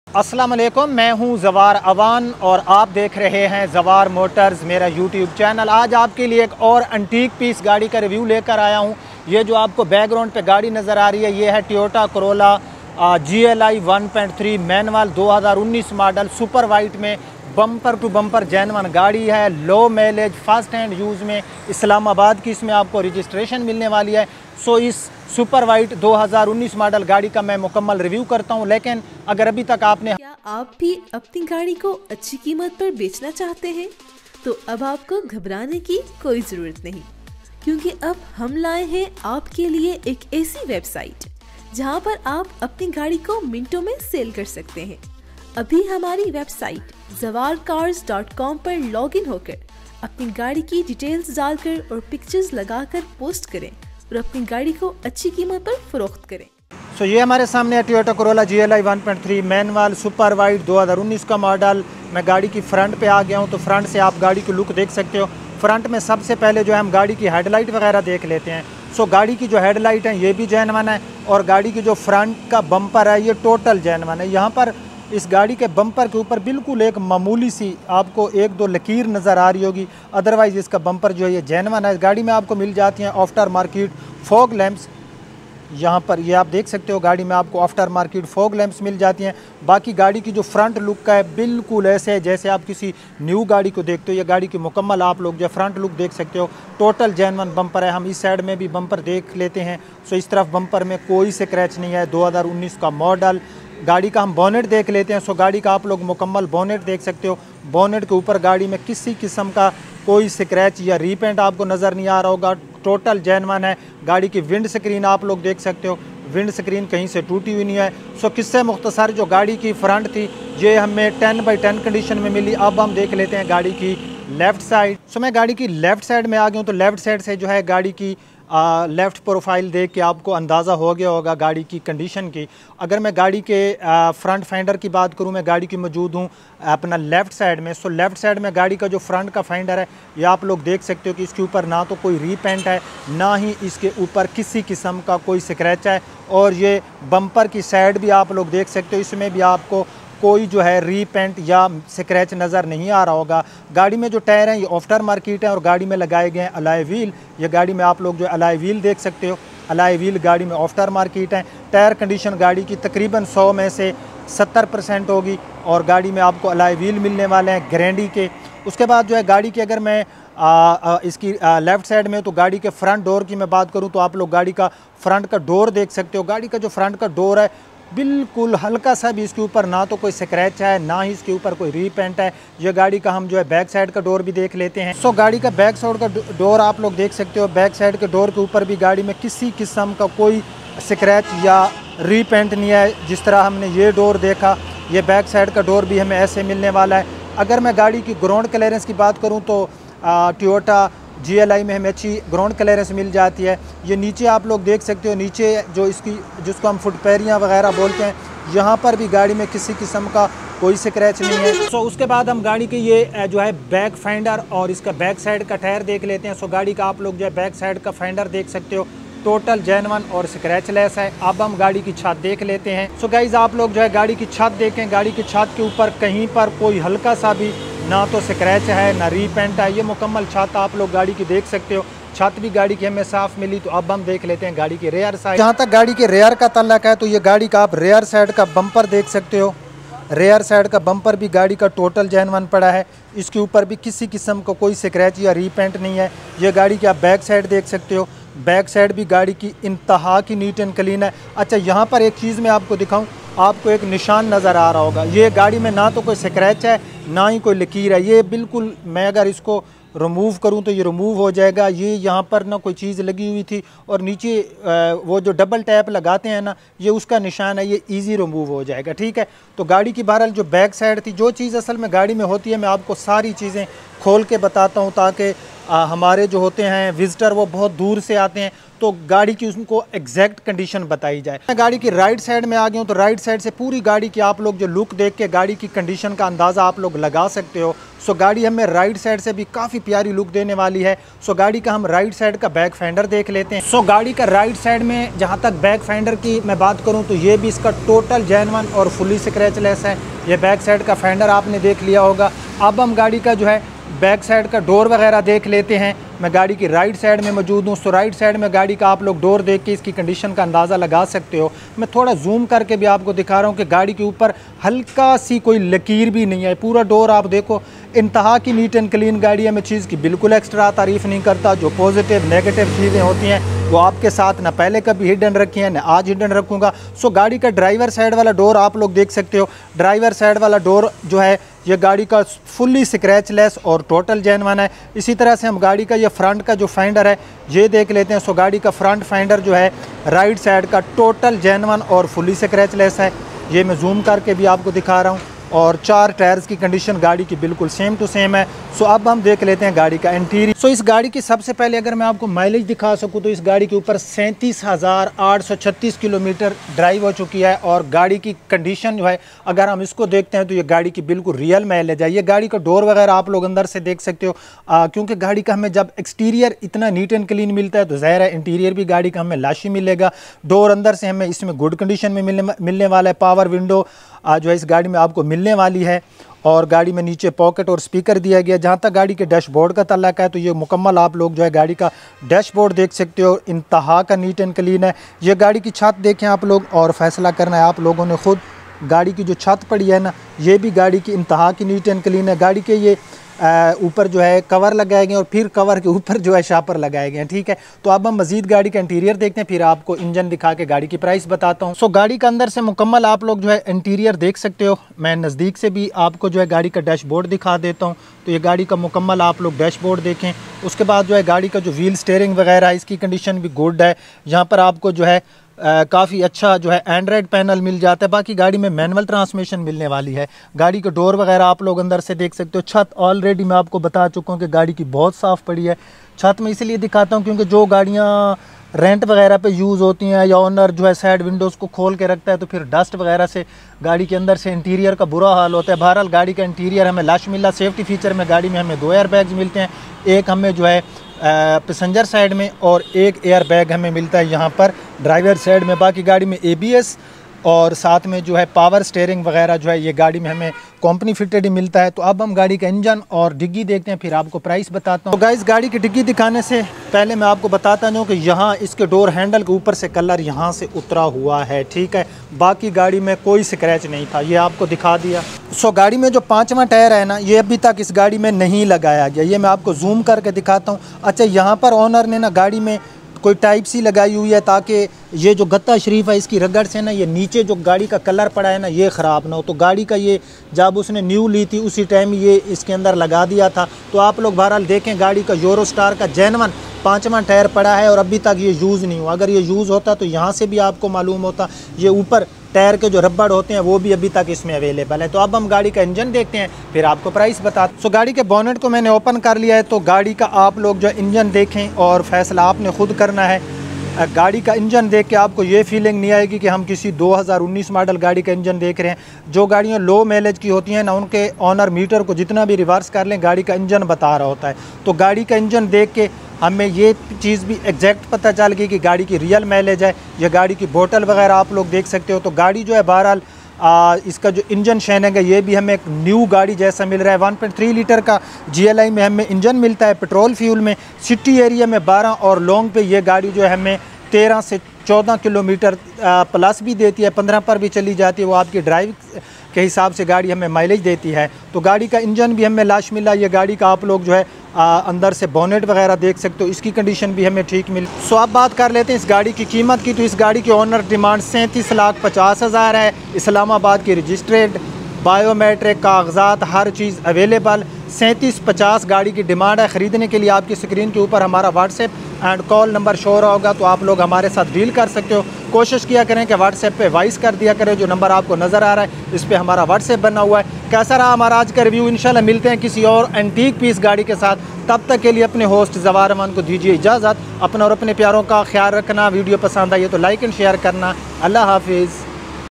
Toyota Corolla | Gli Super White | 1.3 Manual 2019 | Condition Zero masters | Zawar Motors
Toyota Corolla Gli Super White model 2021 Bumper To Bumper Genuine For Sale Complete Review Price
______________/LINKS\_____________________________